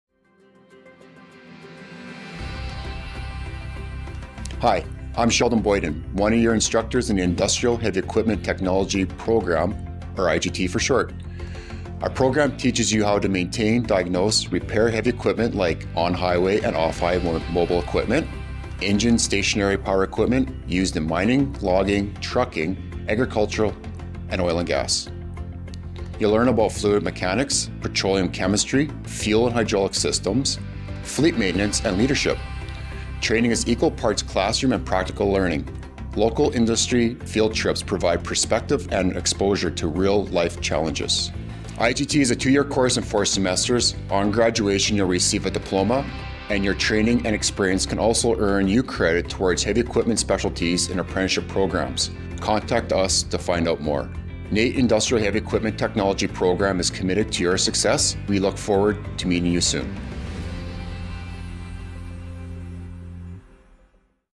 Study Industrial Heavy Equipment Technology at NAIT - Program Overview
The Industrial Heavy Equipment Technology diploma program at NAIT will teach you how to diagnose and repair machinery in the heavy equipment industry competently and safely. The program focuses on highway and off-highway mobile equipment, as well as engine-powered stationary equipment used in mining, logging, trucking, agriculture, gas processing, oilfield servicing and construction.